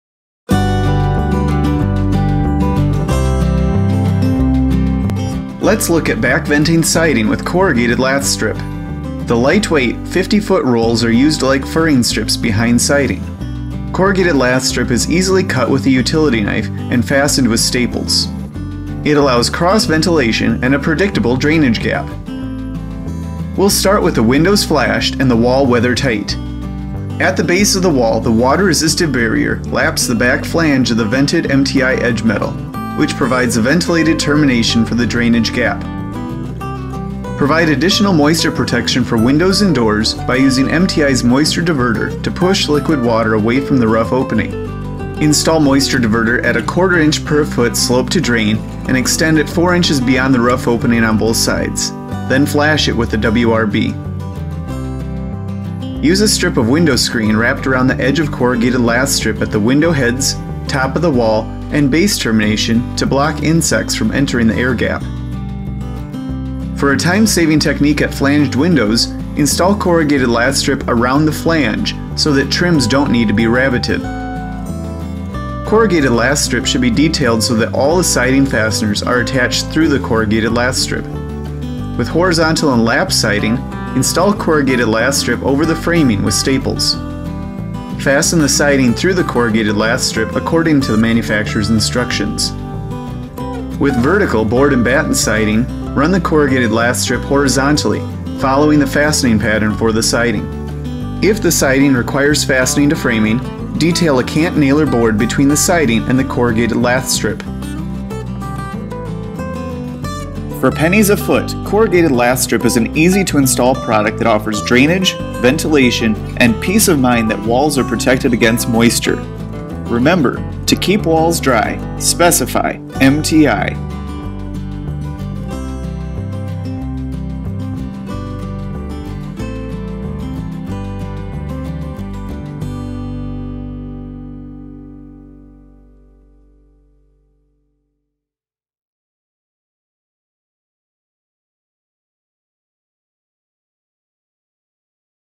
Back-Vent Siding - Rainscreen Furring Strips
Sidings like Hardie Board, LP Smartside, and natural cedar benefit from a rainscreen air gap between the siding and the housewrap. Corrugated Lath Strip creates an air gap that prevents entrapped moisture and helps the siding last longer.

Corrugated Lath Strip is lightweight and easily trimmed with a utility knife. It allows cross-ventilation and free drainage behind siding. Works with lap, tongue and groove, and board and batten siding.